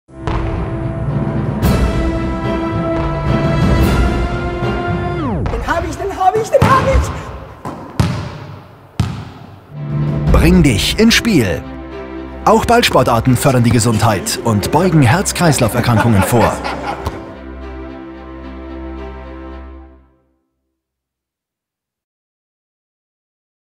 Team Rot-Weiß-Rot Volleyball
Kampagne des Team ROT-WEISS-ROT zur Verbreiterung von populären Ballsportarten.
Der generelle Zugang. Wir nehmen den Grundgedanken „Bring' dich ins Spiel" wortwörtlich und bringen zur Abwechslung ganz normale Menschen „ins Spiel". Das ist einerseits charmant, weil es wieder eine neue Dimension in die Team ROT-WEISS-ROT Kommunikation bringt, zum anderen unterstützt es sehr stark den Anspruch, die Bevölkerung im Zuge der Kampagne zu mehr Bewegung zu motivieren (Social-Legitimierung). Im Jahr 2011 finden in Österreich drei Ballsport-Großveranstaltungen statt. Die Volleyball EM, Faustball-WM und American Football-WM sollten seitens des BMVLS und seines Team ROT-WEISS-ROT Unterstützung erhalten und gleichzeitig ÖsterreicherInnen zu diesen Sportarten bringen. St. Stephen´s GmbH Werbeagentur Wien, Filmproduktion Jenseide.